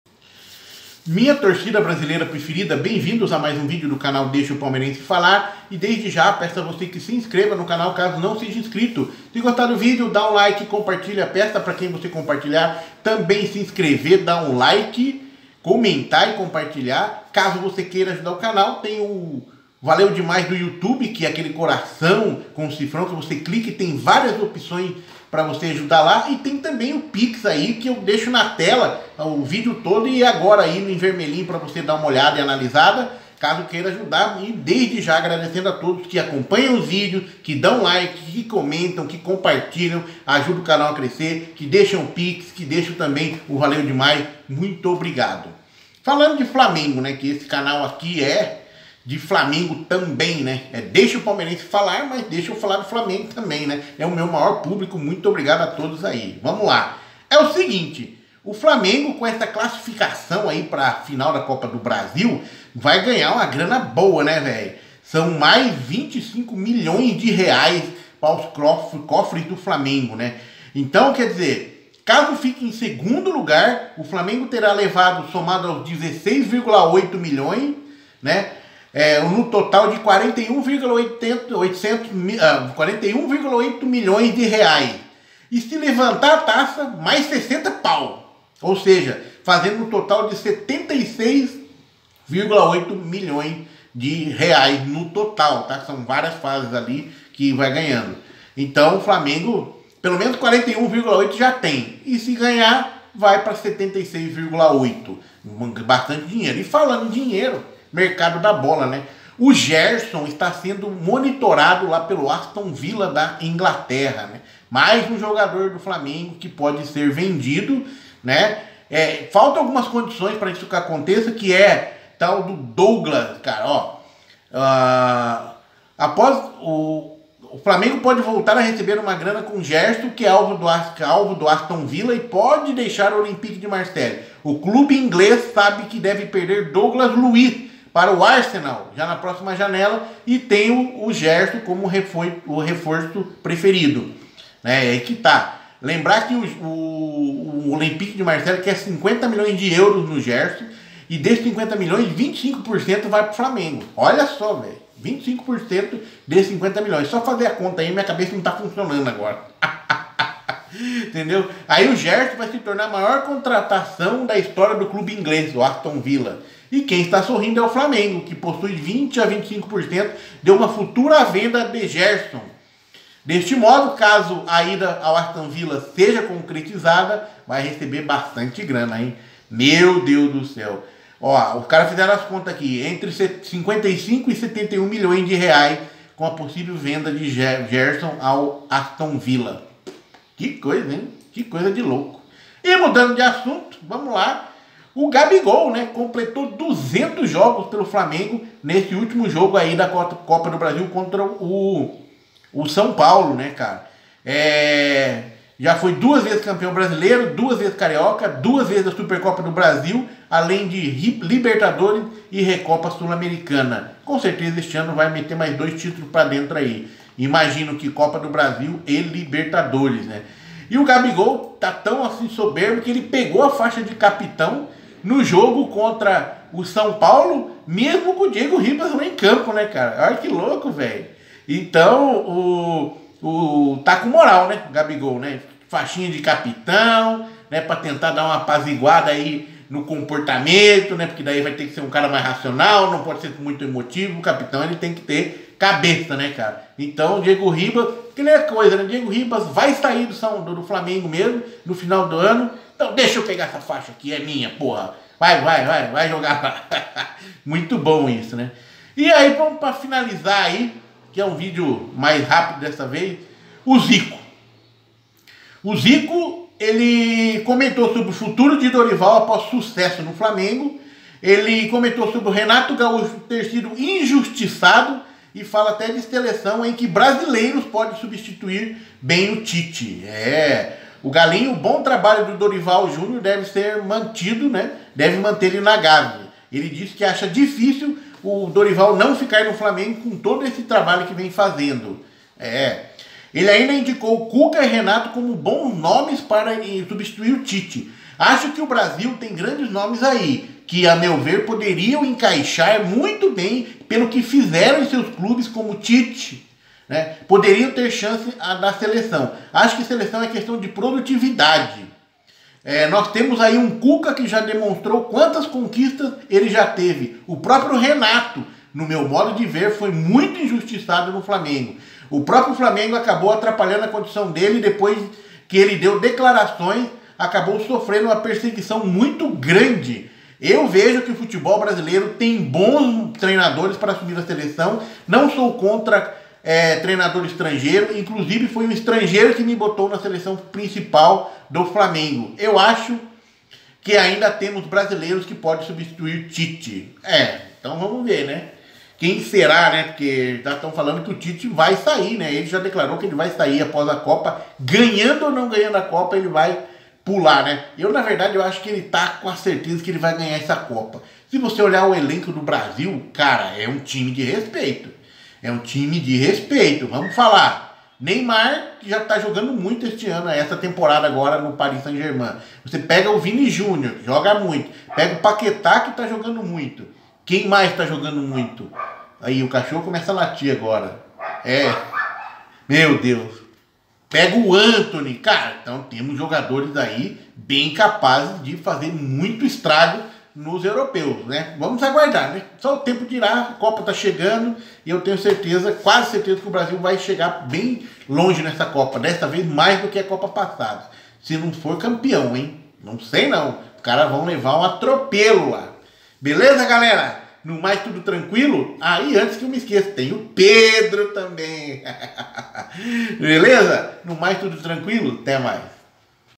ÍDOLO ZICO FALA DE DORIVAL E RENATO. GERSON + $ FLAMENGO. GABIGOL CAPITÃO. COPA DO BRASIL TRAZ + $$.
falando sobre declarações de Zico ex jogador do Flamengo e idolo da torcida sobre Permanência de Dorival Júnior no comando do time de futebol do Flamengo, sobre a injustiça a Renato Gaúcho e sobre possibilidade de treinadores brasileiros assumirem a seleção brasileira após a saída anunciada de Tite Pos copa do mu do 2022 do Qatar.
falando sobre a premiação total que receberá o campeão da copa do Brasil. conversando sobre o percentual de dinheiro de eventual venda de Gerson do Olympique de Marselha ao Aston Villa da PREMIER LEAGUE inglesa.  falando sobre a atitude de GABIGOL de pegar a faixa de capitão do time do flamengo no jogo contra o São Paulo na semi final da copa DO Brasil

Fonte
UOL ESPORTE
Ge.globo.com